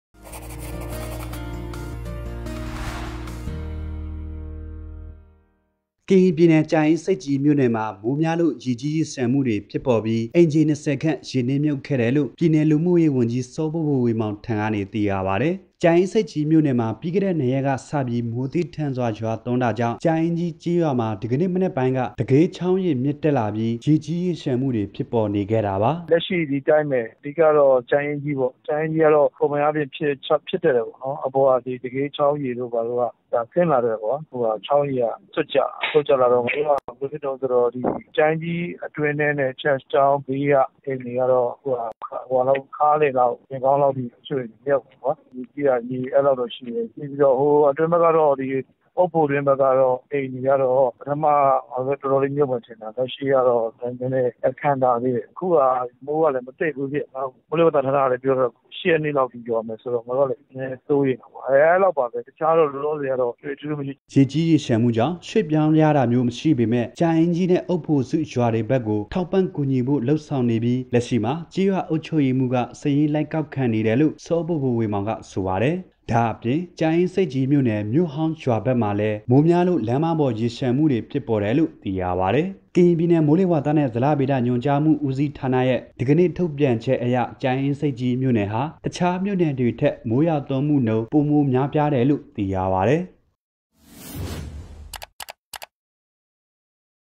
မိုးများလို့ ကြာအင်းဆိပ်ကြီးမှာ အိမ်ခြေတစ်ချို့ ရေမြုပ်
ဩဂုတ်-၃၊ ၂၀၂၀

ကရင်ပြည်နယ် ကြာအင်းဆိပ်ကြီးမြို့နယ်မှာ မိုးများလို့ ရေကြီးရေလျှံမှုတွေဖြစ်ပေါ်ပြီး အိမ်ခြေနှစ်ဆယ်ခန့် ရေနစ်မြုပ်ခဲ့တယ်လို့ ပြည်နယ်လူမှုရေးဝန်ကြီး စောဘိုဘိုဝေမောင်ထံကနေ သိရပါတယ်။

ကြာအင်းဆိပ်ကြီးမြို့နယ်မှာ ပြီးခဲ့တဲ့ နှစ်ရက်က စပြီး မိုးသည်းထန်စွာရွာသွန်းတာကြောင့် ကြာအင်းကြီးကျေးရွာမှာ ဒီကနေ့မနက်ပိုင်းက တဂေးချောင်းရေမြင့်တက်လာပြီး ရေကြီးရေလျှံမှုတွေဖြစ်ပေါ်နေခဲ့တာပါ။

ရေကြီးရေလျှံမှုကြောင့် ရွှေ့ပြောင်းရတာမျိုးမရှိပေမယ့် ကြာအင်းကြီးနဲ့ အုတ်ဖိုစုရွာတွေဘက်ကို ထောက်ပံ့ကူညီဖို့လုပ်ဆောင်နေပြီး လက်ရှိမှာ ကျေးရွာအုပ်ချုပ်ရေးမှူးက စာရင်းလိုက်ကောက်ခံနေတယ်လို့ စောဘိုဘိုဝေမောင်ကဆိုပါတယ်။

ဒါ့အပြင် ကြာအင်းဆိပ်ကြီးမြို့နယ် မြို့ဟောင်းရွာဘက်မှာလည် းမိုးများလို့ လမ်းမပေါ် ရေလျှံမှုတွေဖြစ်ပေါ်တယ်လို့လည်း သိရပါတယ်။

ကရင်ပြည်နယ် မိုးလေဝသနဲ့ ဇလဗေဒညွှန်ကြားမှုဦးစီးဌာနရဲ့  ဒီကနေ့ထုတ်ပြန်ချက်အရ ကြာအင်းဆိပ်ကြီးမြို့နယ်ဟာ တခြားမြို့နယ်တွေထက် မိုးရွာသွန်းမှုနှုန်း ပိုမိုများပြားတယ်လို့ သိရပါတယ်။